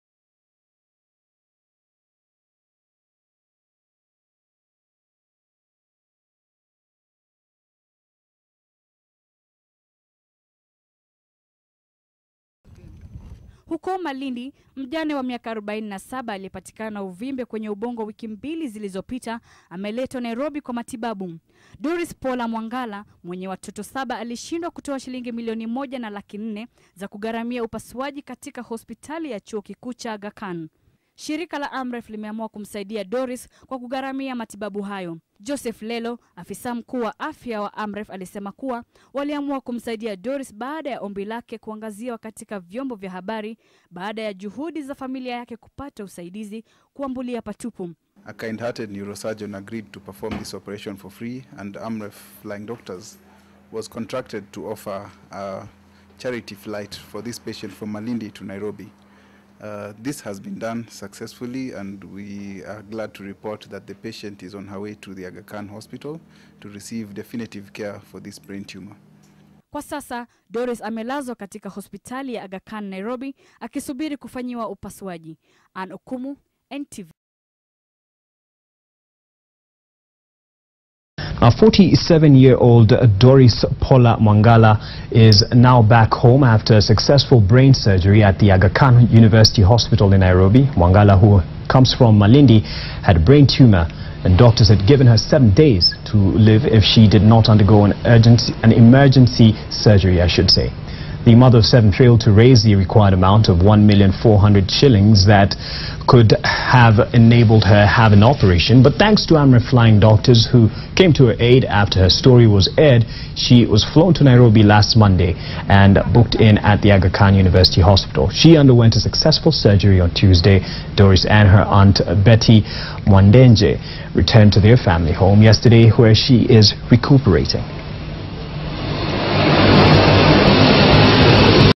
AMREF Flying Doctors evacuates a brain tumour patients from Malindi to Nairobi
On Tuesday 27th October, Doris Paula Mwangala a 47 years old woman whose medical condition was leaked from the newspaper was airlifted by AMREF Flying Doctors from Malindi to Nairobi for specialised management of a brain tumour.